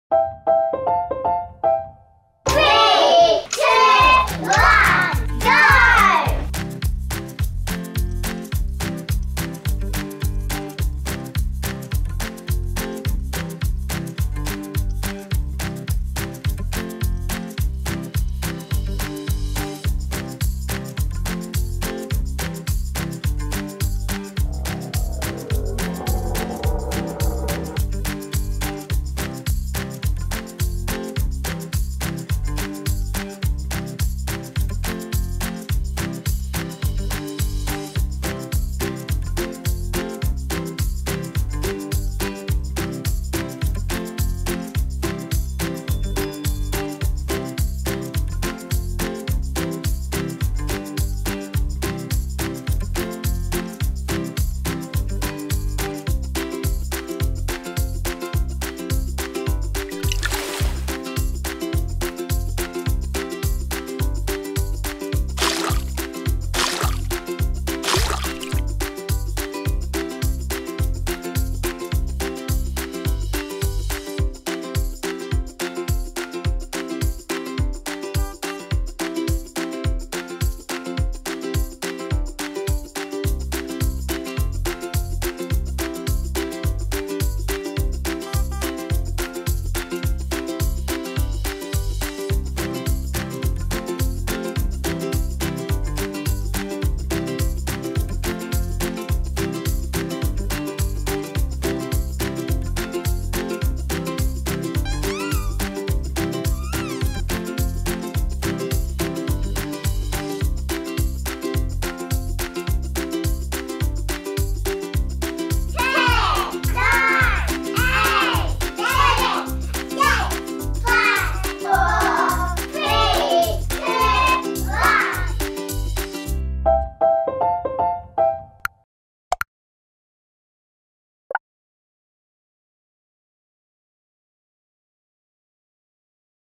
2 Minute Fun House Timer - Kids Visual Timer
Fun house Timer! Watch as the house builds up with different fun and colourful rooms. Watch out for 5 surprises making their way through.

Sound FX from Zapsplat.com & Freesound.org
Unicorn, Bubbles & Teddy Bear Sound Effect from Pixabay